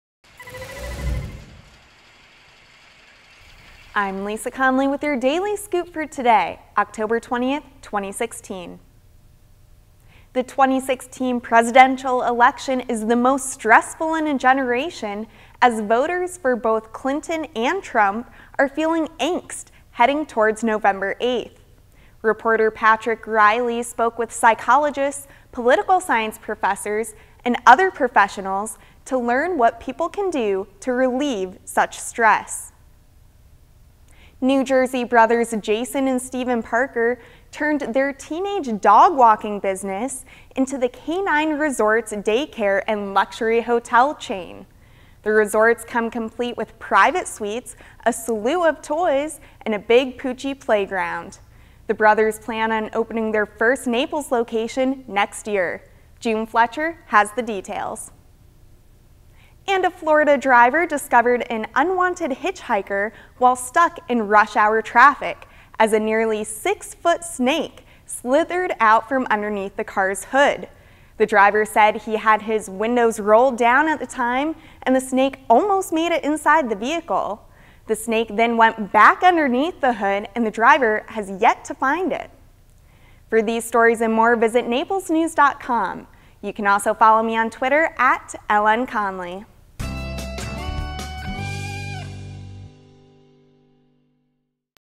Daily Scoop for Oct 20, 2016
THE 2016 PRESIDENTIAL ELECTION IS THE MOST STRESSFUL IN A GENERATION AS VOTERS FOR BOTH CLINTON AND TRUMP ARE FEELING ANGST HEADING TOWARD NOV. 8. REPORTER PATRICK RILEY SPOKE WITH PSYCHOLOGISTS, POLITICAL SCIENCE PROFESSORS AND OTHER PROFESSIONALS TO LEARN WHAT PEOPLE CAN DO TO RELIVE SUCH STRESS.
NEW JERSEY BROTHERS JASON AND STEVEN PARKER TURNED THEIR TEENAGE DOG-WALKING BUSINESS INTO THE K-9 RESORTS DAYCARE AND LUXURY HOTEL CHAIN. THE RESORTS COME COMPLETE WITH PRIVATE SUITES, A SLEW OF TOYS AND A BIG POOCHIE PLAYGROUND. THE BROTHERS PLAN ON OPENING THEIR FIRST NAPLES LOCATION NEXT YEAR. JUNE FLETCHER HAS THE DETAILS.
AND A FLORIDA DRIVER DISCOVERED AN UNWANTED HITCHIKER WHILE STUCK IN RUSH HOUR TRAFFIC AS A NEARLY SIX-FOOT SNAKE SLITHERED OUT FROM UNDERNEATH THE CAR’S HOOD. THE DRIVER SAID HE HAD HIS WINDOWS ROLLED DOWN AT THE TIME AND THE SNAKE ALMOST MADE IT INSIDE THE VEHICLE. THE SNAKE THEN WENT BACK UNDERNEATH THE HOOD AND THE DRIVER HAS YET TO FIND IT.